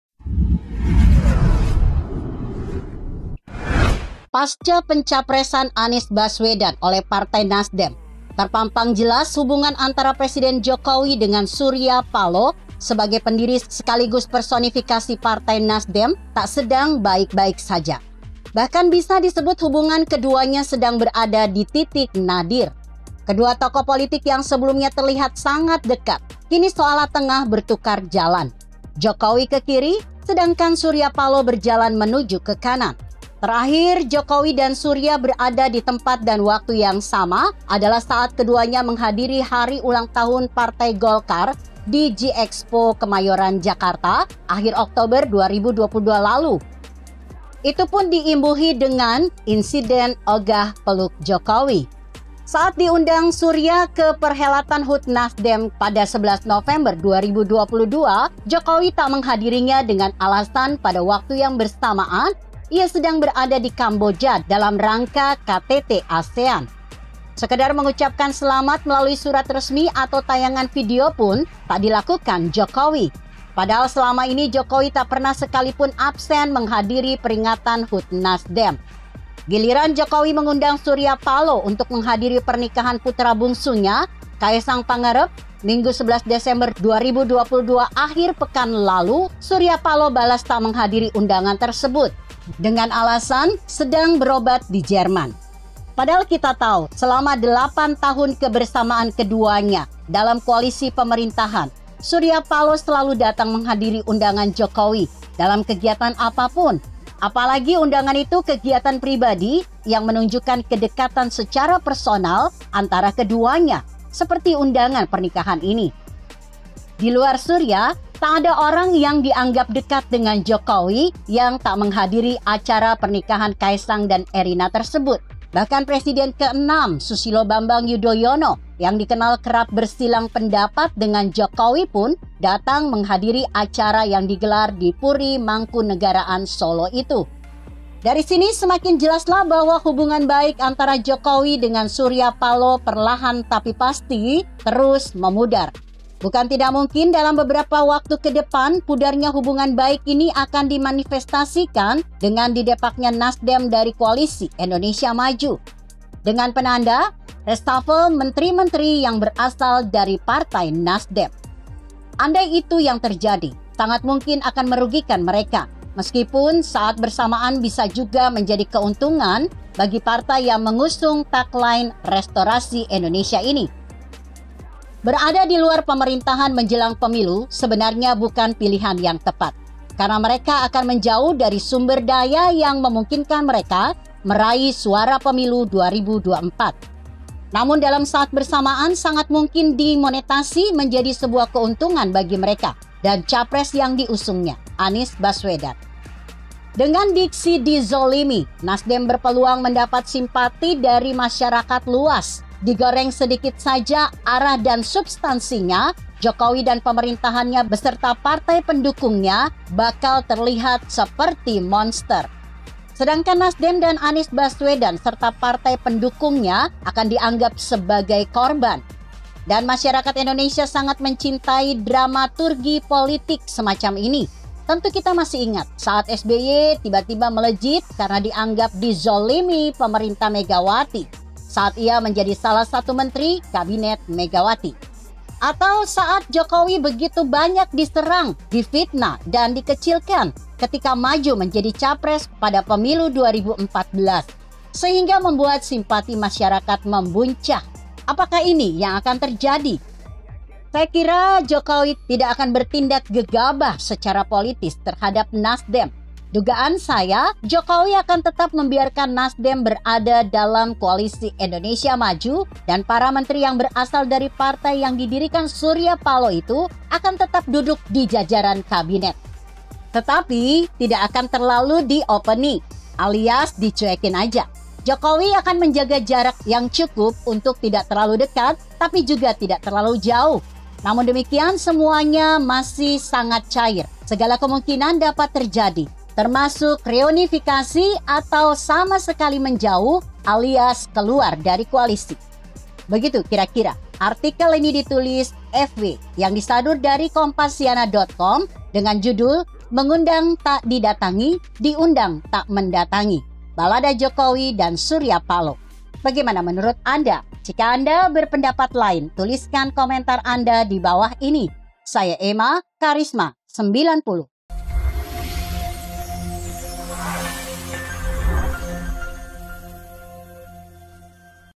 Mengundang Tak Didatangi, Diundang Tak Mendatangi, Balada Jokowi dan Surya Paloh
Kedua tokoh politik yang sebelumnya terlihat sangat dekat, kini seolah tengah bertukar jalan. Jokowi ke kiri, sedangkan Surya Paloh berjalan menuju ke kanan.

Terakhir Jokowi dan Surya berada di tempat dan waktu yang sama adalah saat keduanya menghadiri hari ulangtahun Partai Golkar di JIEXPO Kemayoran, Jakarta, akhir Oktober 2022 lalu. Itu pun diimbuhi dengan "insiden ogah peluk Jokowi."

Saat diundang Surya ke perhelatan HUT Nasdem pada 11 November 2022, Jokowi tak menghadirinya dengan alasan pada waktu yang bersamaan ia sedang berada di Kamboja dalam rangka KTT Asean.

Konten ini telah tayang di Kompasiana.com dengan judul "Mengundang Tak Didatangi, Diundang Tak Mendatangi, Balada Jokowi dan Surya Paloh", Klik untuk baca:

Kreator: Efwe

Tulis opini Anda seputar isu terkini di Kompasiana.com